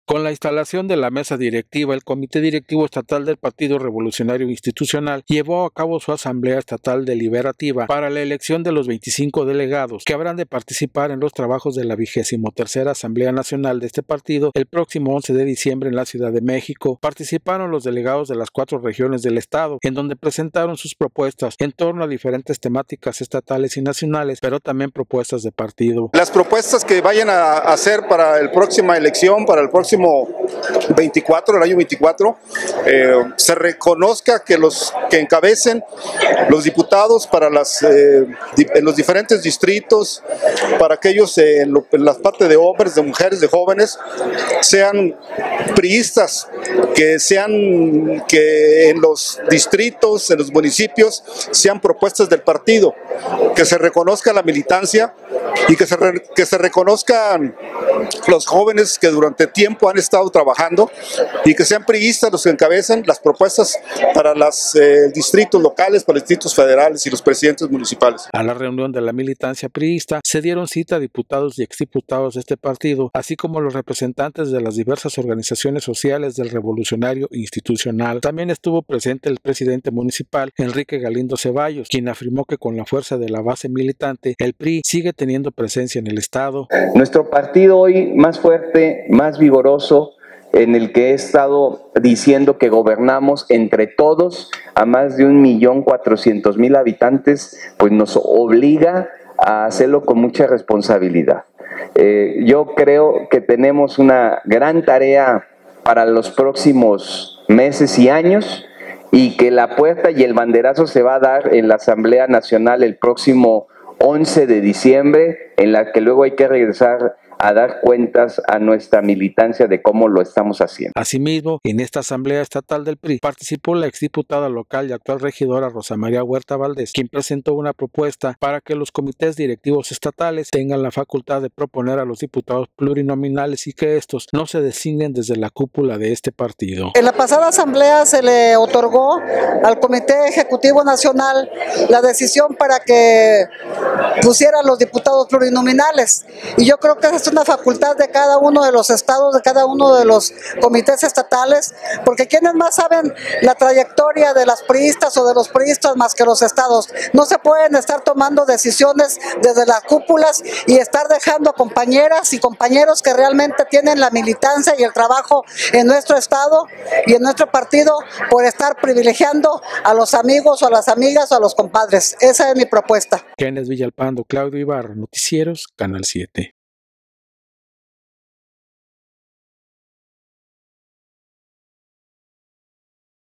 CDE PRI llevó a cabo asamblea estatal para elegir a nuevos delegados.
SLP.- Con la instalación de la Mesa Directiva el Comité Directivo Estatal del Partido Revolucionario Institucional llevó a cabo su Asamblea Estatal deliberativa, para la elección de los 25 delegados que habrán de participar en los trabajos de la Vigésimo Tercera Asamblea Nacional de este partido, el próximo 11 de diciembre en la Ciudad de México.

Participaron los delegados de las 4 regiones del estado en donde presentaron sus propuestas en torno a diferentes temáticas estatales y nacionales, pero también propuestas de partido.

A la reunión de la militancia priísta se dieron cita diputados y exdiputados de este partido asi como los representantes dc las diversas organizaciones sociales del Revolucionario Institucional.

También estuvo presente el presidente municipal Enrique Galindo Ceballos quien afirmó que con la fuerza de la base militante el partido sigue teniendo presencia en el estado.

Asimismo, en esta Asamblea Estatal del PRI participó la exdiputada local y actual regidora Rosa María Huerta Valdés, quien presentó una propuesta para que los Comités Directivos Estatales, tengan la facultad de proponer a los diputados plurinominales y que éstos, no se designen desde la cúpula de este partido.